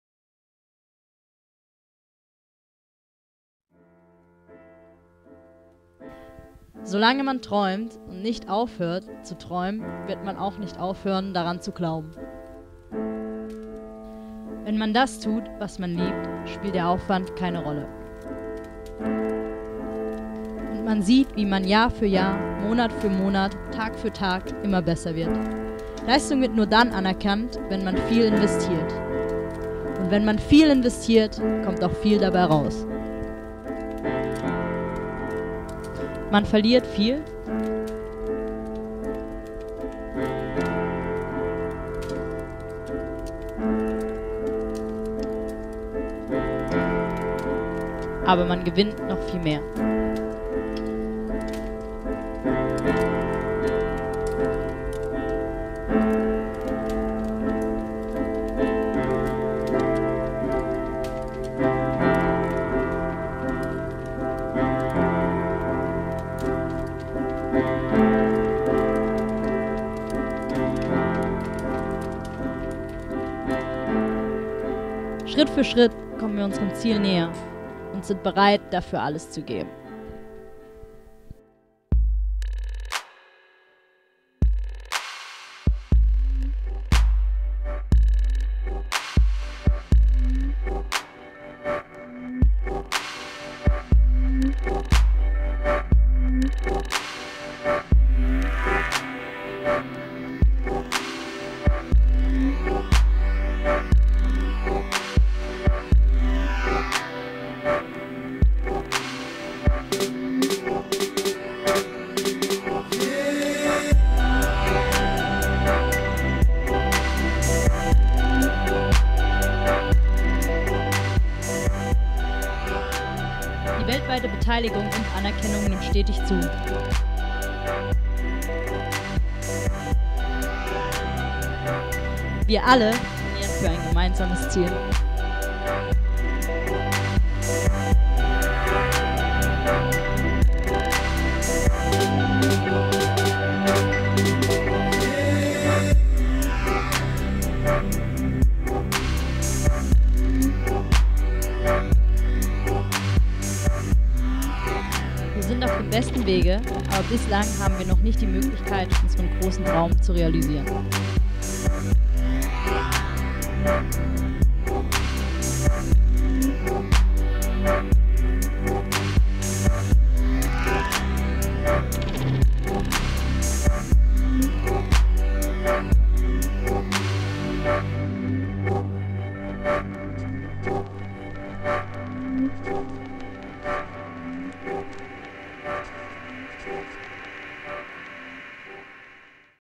REALIZE OUR DREAM - Trailer
Realize our Dream!

In our todays Canoe-Slalom world one discipline is definitely missing. C1 women will only have the chance to get involved into the Olympic program of Rio 2016 if especially the IOC find an suitable and equating solution.
We as a constantly growing and developing discipline want to show the world: We are able to develop to an Olympic level! Our number of international and different number of participants is growing in an enormous speed every new season! A lot of young girls all over the world are starting to compete in this category. And having a lot fun with it.
It is out of question that we just want to have the chance to proof this all on the highest international platform as an Olympic discipline. This Trailer is ought to show that without any doubt we are able to participate in the Olympic games and that we are athletes as any others!
A big movie will follow in 2013. Until this day. Support us as athletes, support C1 woman as an Olympic discipline and as the main fact:
SUPPORT OUR DREAM!

In unserer heutigen Kanuslalom-Welt findet man eine wichtige Disziplin heutzutage immer noch nicht. C1 weiblich wird solange keine Chance bekommen, in das olympische Programm aufgenommen zu werden, bis ICF und IOC nicht eine mögliche Lösung gefunden haben.
Wir als aufstrebende Disziplin wollen der Welt zeigen: Wir haben ein olympisches Level! Wir sind ausreichend viele! Und wir haben es verdient, eine olympische Disziplin zu sein!
Dieser Trailer soll allen Menschen zeigen, die an diesem Punkt zweifeln, dass wir das Recht haben, an den Olympischen Spielen 2016 in Rio teilzunehmen und dass wir Athleten sind, wie jeder Andere!

Der große Film wird 2013 folgen. Bis dahin: Unterstützt uns als Athleten, unterstützt C1 weiblich als olympische Disziplin und vor allem:
UNTERSTÜTZT UNSEREN TRAUM!